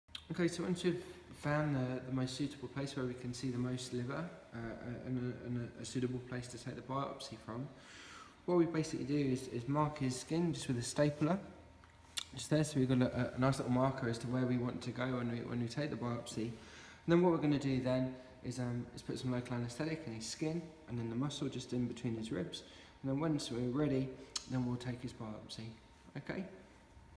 Equine Liver biopsy Part 4
This series of short videos shows how we take a liver biopsy in a horse in order to investigate the cause of liver disease.